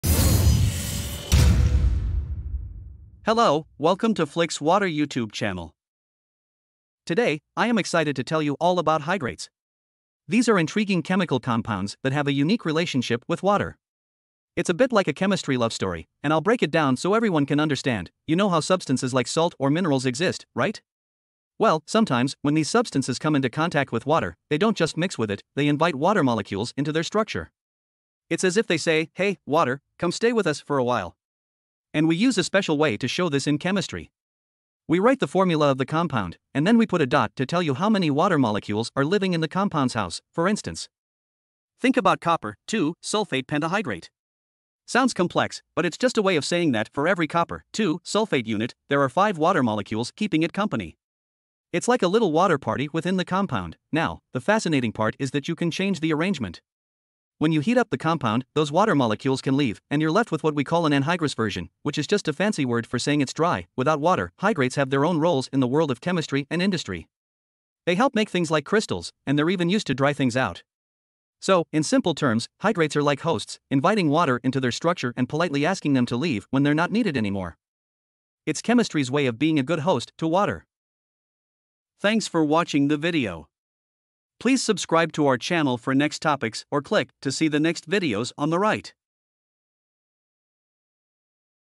What Is A Hydrate?
Hydrates have their own roles in the world of chemistry and industry. They help make things like crystals, and they're even used to dry things out. So, in simple terms, hydrates are like hosts, inviting water into their structure and politely asking them to leave when they're not needed anymore. It's chemistry's way of being a good host to water!